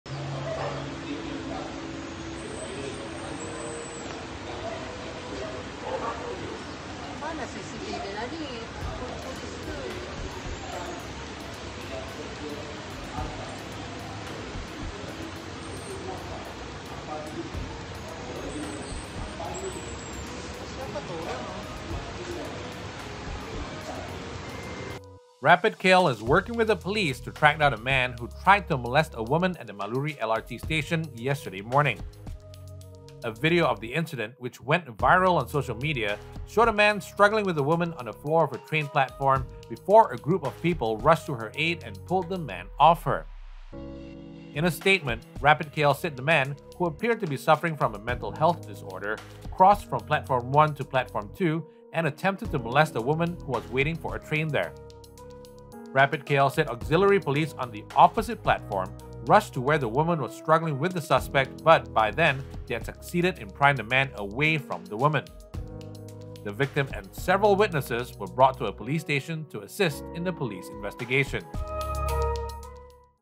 RapidKL working with cops to catch Maluri LRT station molester
RapidKL says the man appeared to be suffering from a mental health disorder.